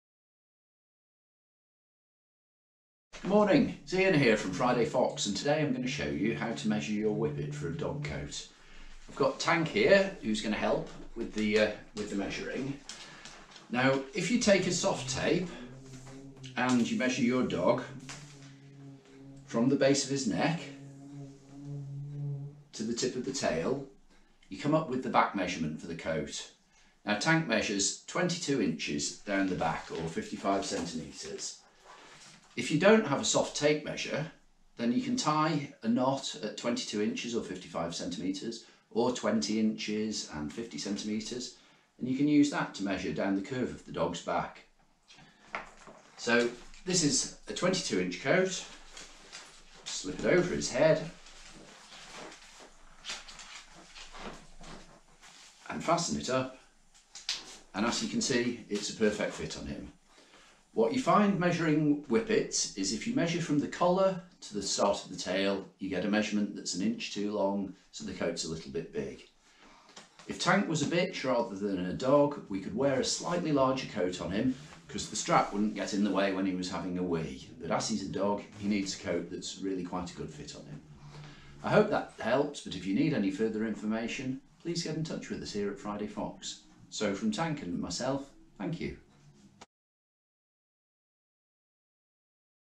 How to measure your whippet for a coat
A short video from Friday Fox to help with measuring whippets for their new coats.